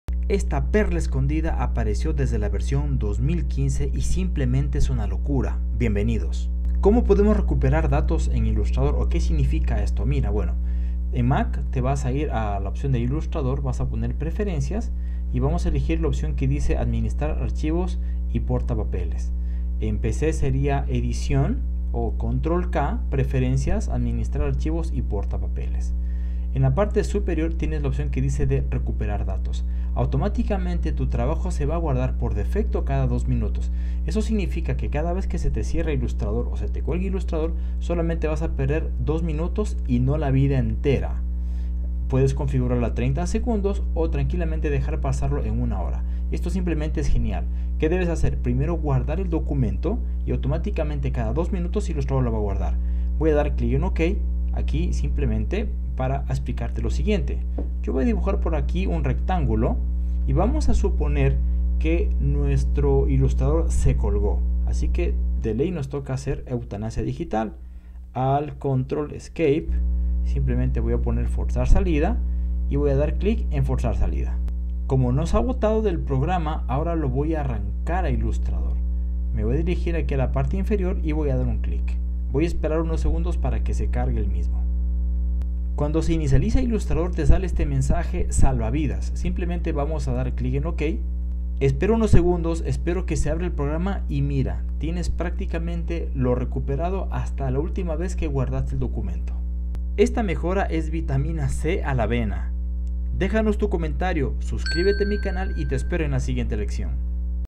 ✅✅✅ Como recuperar archivo no guardado Adobe Illustrator
Como recuperar archivo no guardado Adobe Illustrator. Si accidentalmente se cerró ilustrador, se colgó y dejó de funcionar? Se fue la luz?. Cómo recuperar archivos no guardados en Adobe Illustrator, desde ahora es posible con la opción de autoguardado. Aplausos para el equipo de programación de Adobe Illustrator. Recuperar archivos y autoguardado en Adobe Illustrator
El presente video tiene como referencia la traducción original de la página de Adobe, para sugerencias escribir de manera directa